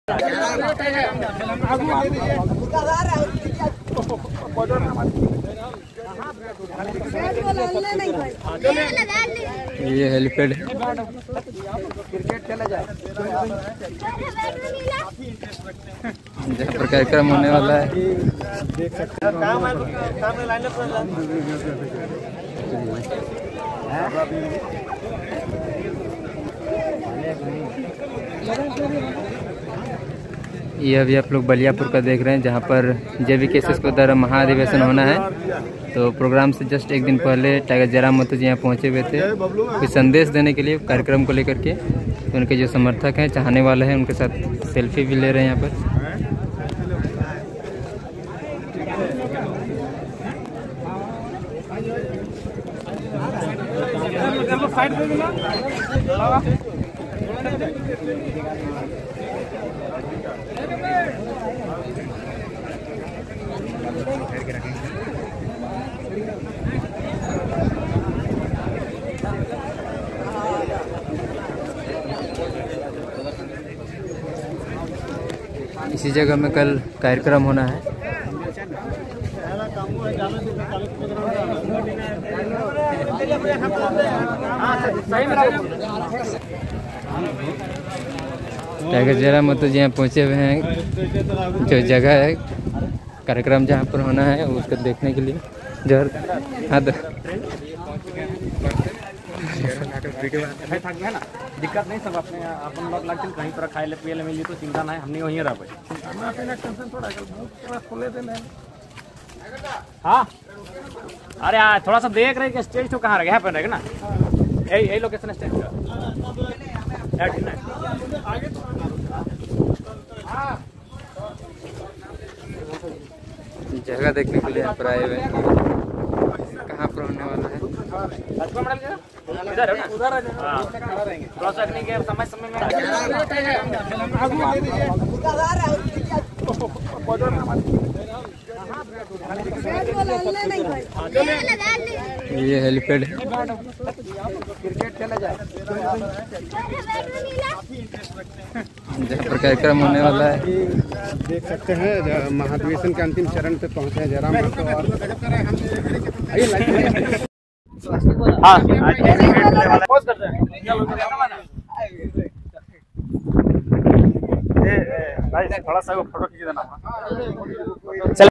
टाइगर जयराम महतो निरीक्षण करने आए बिनोद धाम धनबाद | Tiger Jairam Mahto Jharkhand Dhanbad Viral Video
टाइगर जयराम महतो निरीक्षण करने आए बिनोद धाम धनबाद | Tiger Jairam Mahto Jharkhand Dhanbad Viral Video @TigerJairamKrantikari